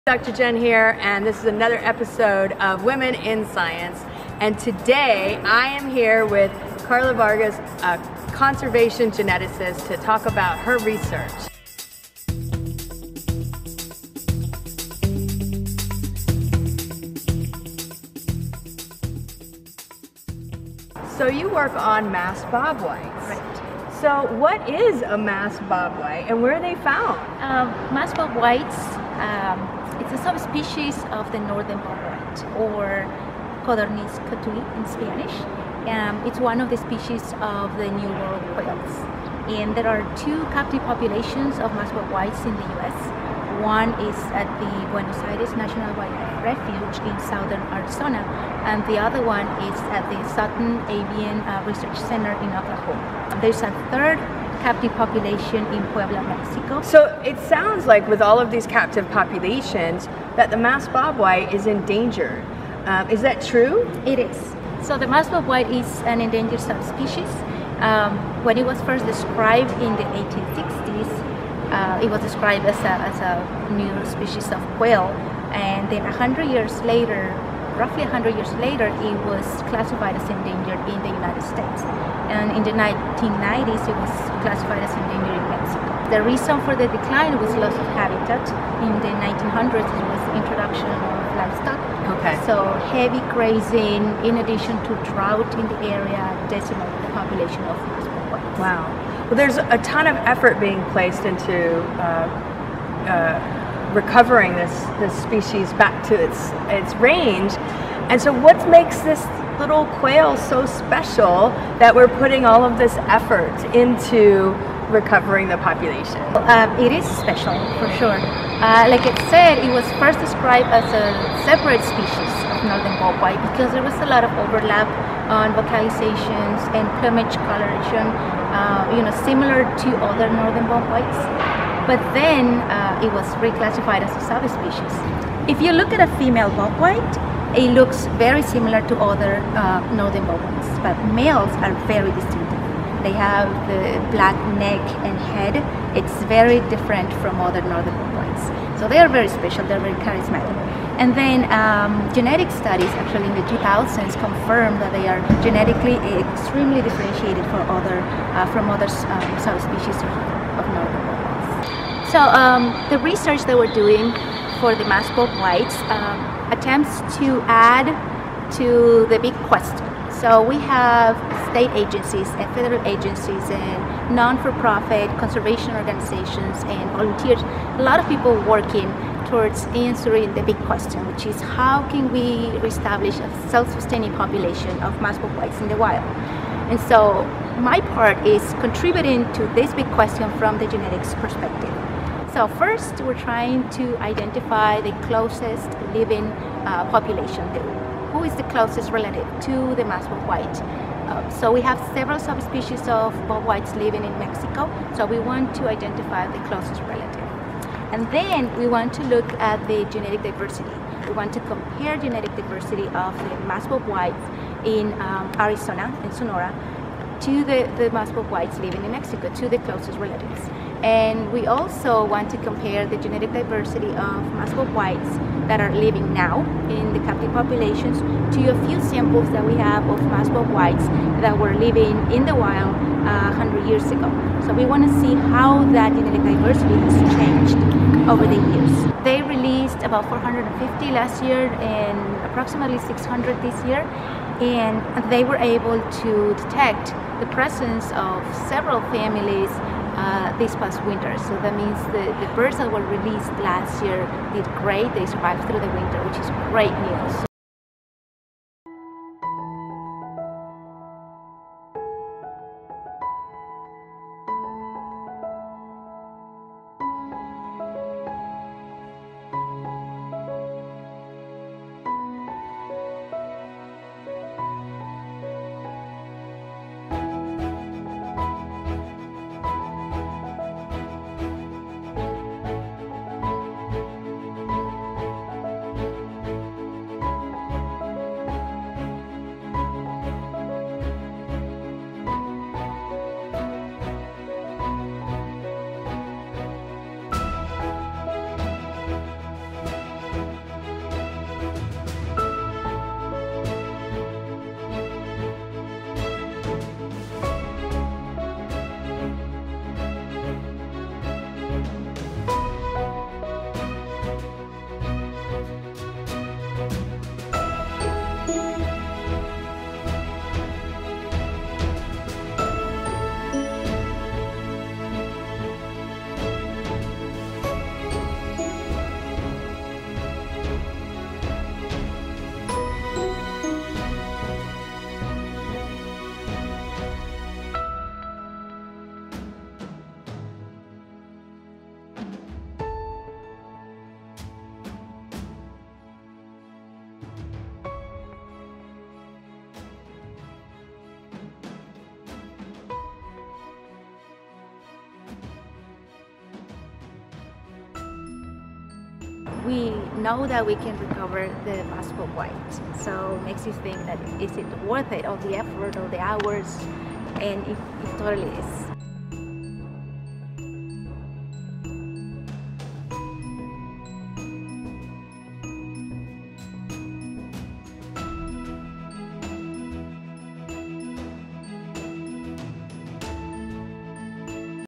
Saving A Species: The Masked Bobwhite
In this episode of Women in Science, I talk with conservation geneticist, Karla Vargas, about her work that is helping to bring the Masked bobwhite quail back from extinction in the wild. Special thanks to Rebecca Chester (Quail Biologist at the  Buenos Aires National Wildlife Refuge), National Park Service Interns: Jesús Dueñas, Maya Tainatongo and Julia Jones, and field biologist,  Ed Kuklinski.

Video and photo Credits:
Footage and photos of Buenos Aires National Wildlife Refuge, pens, and Masked bobwhite release: Karla Vargas
Other footage: Wild Connection Media
Photo of Masked bobwhite: USFWS Public Domain